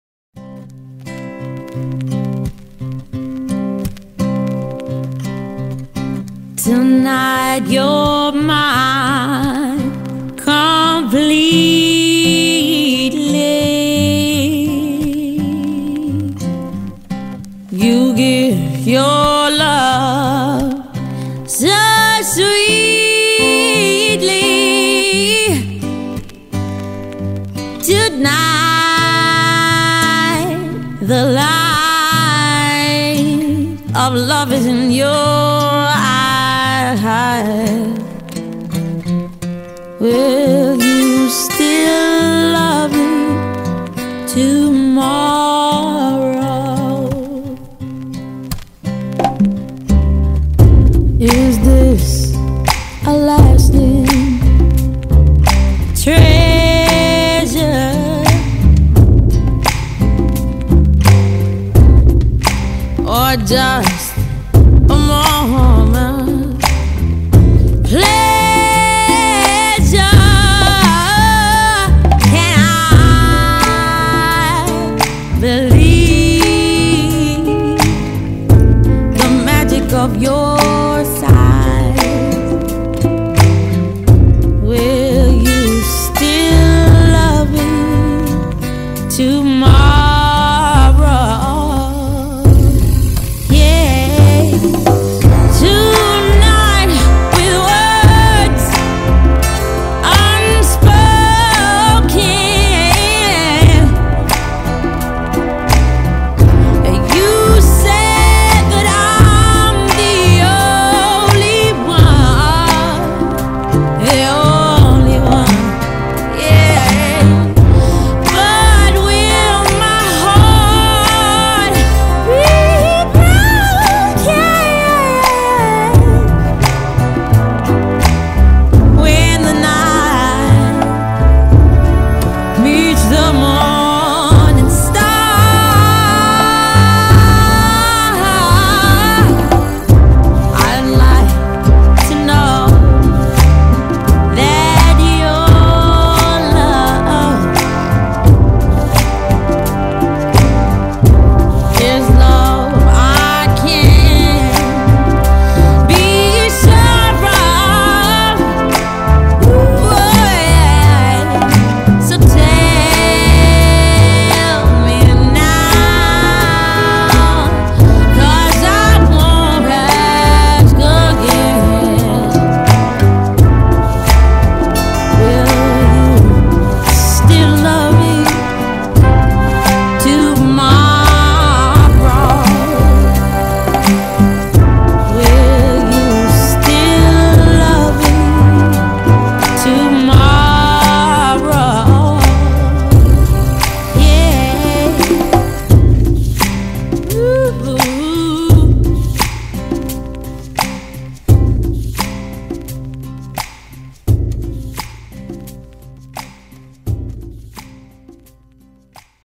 Plener Niny i Mariusza
Plener ślubny nagrany w Arkadii i w Płocku.